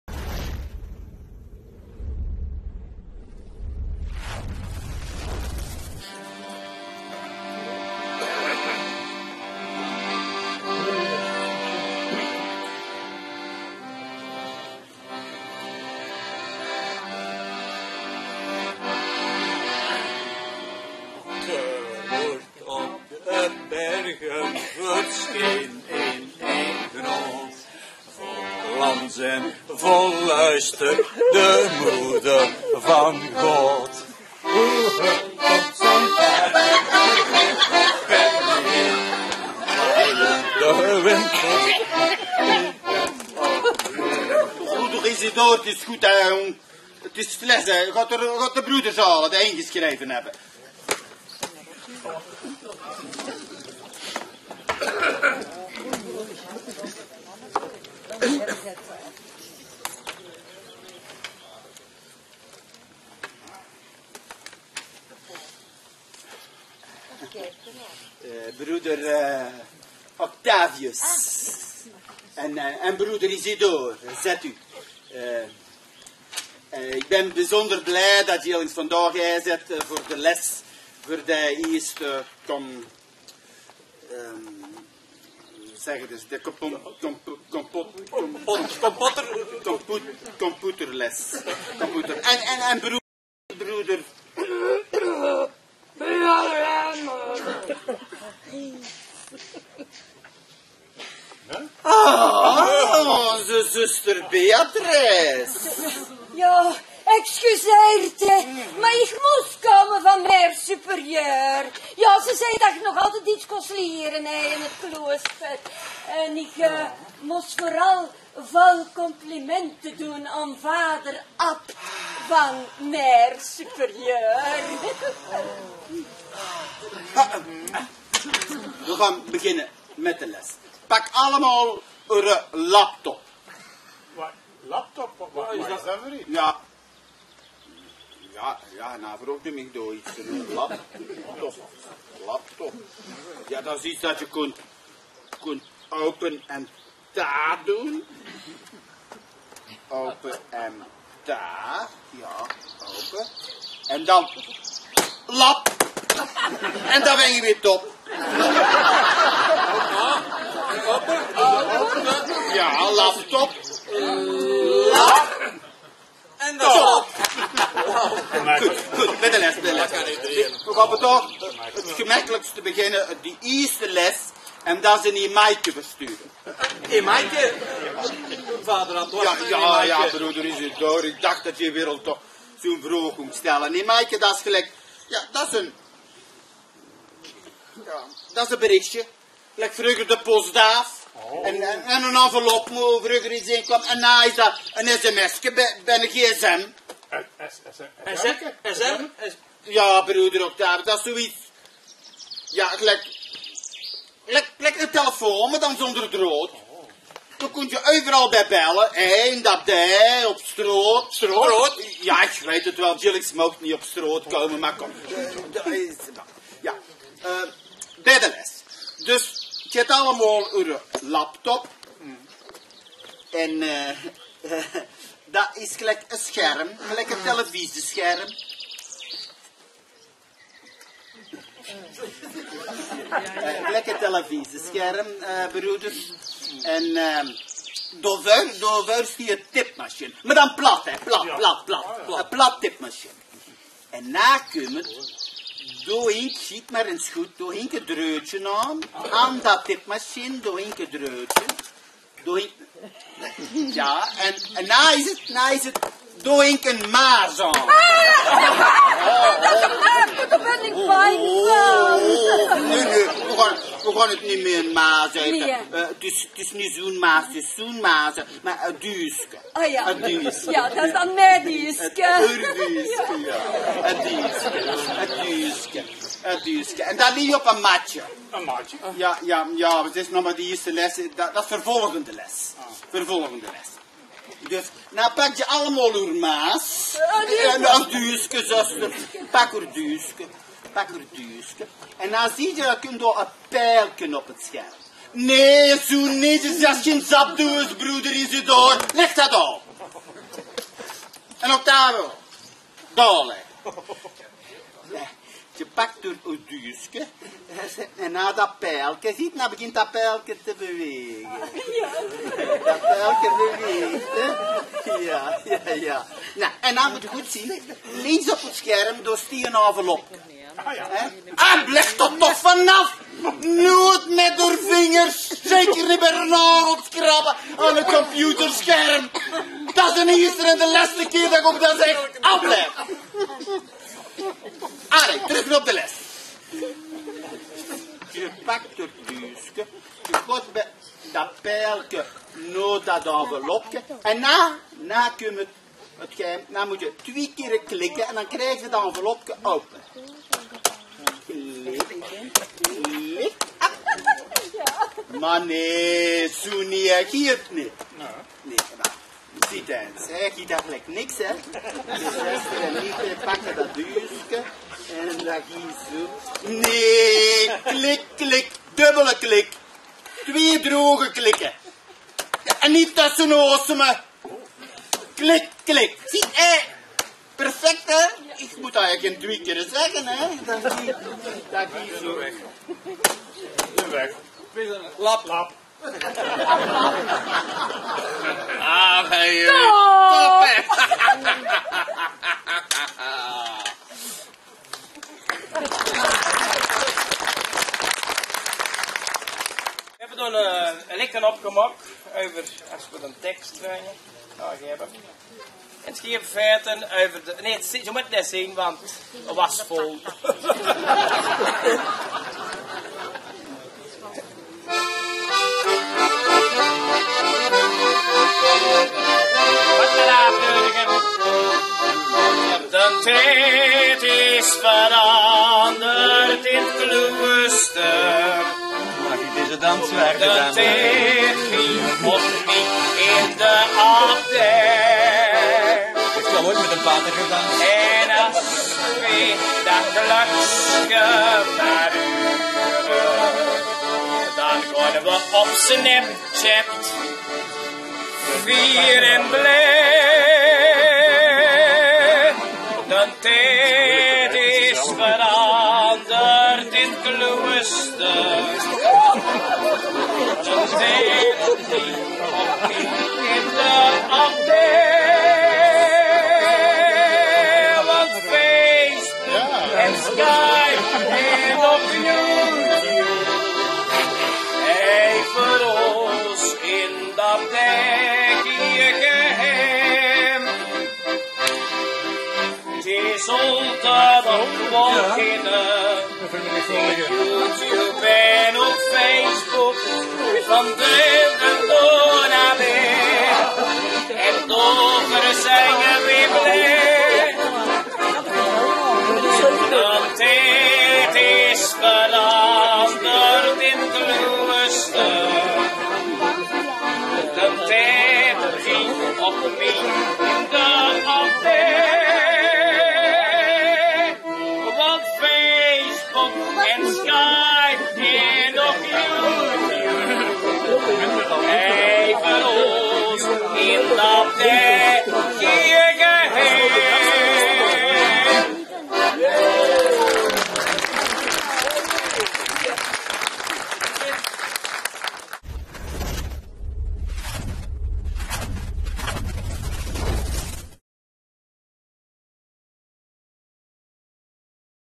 De Boddelkèèr 2016 De abt van Sint Truiden en het kloosterleven
De Boddelkèèr heeft haar tenten opgebroken en het wordt weer een jaartje wachten  op hun nieuwe fratsen. Dan brengen we de winter toch gewoon door met nagenieten!
De Bòddelkèèr is een mix van historie, actualiteit, anekdotes, maatschappijkritiek en vooral … humor! En dit alles in het “plat Sintrùins”.  ’t Is ver niks & brènk oer stulke mie”.